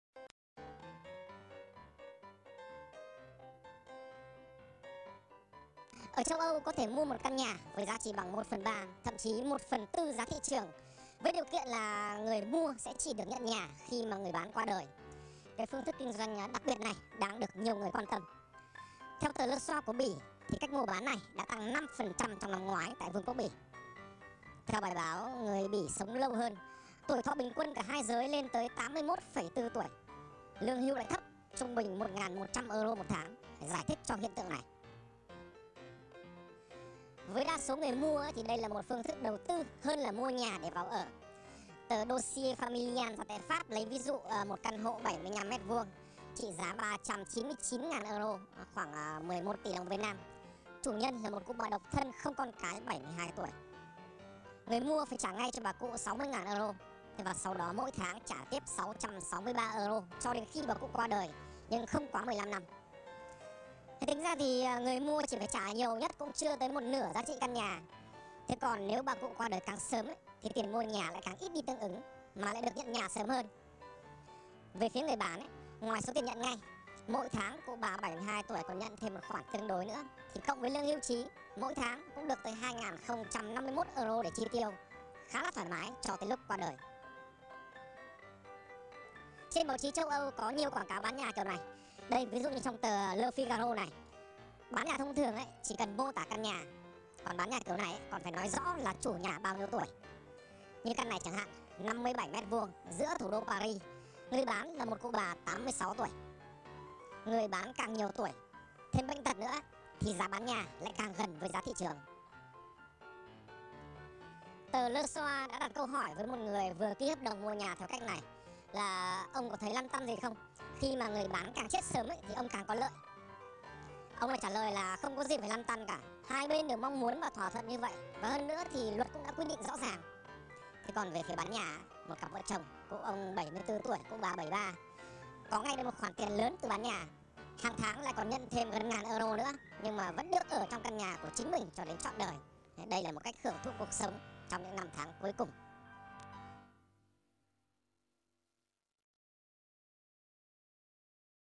Mua nhà của người sống- nhận nhà từ người c.hết để tiết kiệm bộn tiền mua nhà
Ở châu Âu có thể mua một căn nhà với giá chỉ bằng 1/3 thậm chí 1/4 giá thị trường với điều kiện là, người mua sẽ chỉ được nhận nhà khi người bán qua đời.
Phương thức kinh doanh đặc biệt này đang được nhiều người quan tâm.
Cách mua bán này đã tăng 5% trong năm ngoái tại vương quốc Bỉ.
Người Bỉ sống lâu hơn, tuổi thọ bình quân cả hai giới lên tới 81,4 tuổi, Lương hưu lại thấp, trung bình 1100 euro một tháng, giải thích cho hiện tượng này.
Với đa số người mua thì đây là một phương thức đầu tư hơn là mua nhà để vào ở.
Ví dụ một căn hộ 75 m vuông trị giá 399 ngàn euro, khoảng 11 tỉ đồng Việt Nam
Chủ nhân là một cụ bà độc thân không con cái 72 tuổi.
Người mua phải trả ngay cho bà cụ 60 ngàn euro
Sau đó mỗi tháng trả tiếp 663 euro cho đến khi bà cụ qua đời, Nhưng không quá 15 năm.
Tính ra thì người mua chỉ phải trả nhiều nhất cũng chưa tới một nửa giá trị căn nhà.
Nếu bà cụ qua đời càng sớm, tiền mua nhà lại càng ít đi tương ứng, Mà lại được nhận nhà sớm hơn.
Về phí người bán, Ngoài số tiền nhận ngay, mỗi tháng cụ bà 72 tuổi còn nhận thêm một khoản tương đối nữa, Cộng với lương hưu trí, mỗi tháng cũng được tới 2051 euro để chi tiêu.
Khá là thoải mái cho tới lúc qua đời.
Trên báo chí châu Âu có nhiều quảng cáo bán nhà kiểu này.
Bán nhà thông thường chỉ cần mô tả căn nhà
Còn bán nhà kiểu này, còn phải nói rõ chủ nhà bao nhiêu tuổi.
Như căn này chẳng hạn, 57 m vuông, giữa thủ đô Paris.
Người bán là một cụ bà 86 tuổi.
Người bán càng nhiều tuổi, thêm bệnh tật nữa thì giá bán nhà càng gần với giá thị trường.
Đặt câu hỏi với một người vừa ký hợp đồng mua nhà theo cách này.
Ông có thấy lăn tăn gì không khi người bán càng chết sớm thì ông càng có lợi?
Ông này trả lời là không có gì phải lăn tăn cả.
Hai bên đền mong muốn và thỏa thuận như vậy. Và hơn nữa, luật cũng đã quy định rõ ràng.
Về phía bán nhà, một cặp vợ chồng, cụ ông 74 tuổi, cụ bà 73
Có ngay được một khoản tiền lớn từ bán nhà, hàng tháng lại còn nhận thêm gần 1000 euro nữa.
Nhưng vẫn được ở trong căn nhà của chính mình cho đến trọn đời.
Đây là một cách hưởng thụ cuộc sống trong những năm tháng cuối cùng.
BCCyoutube -  Quy Nhon, 1/11/18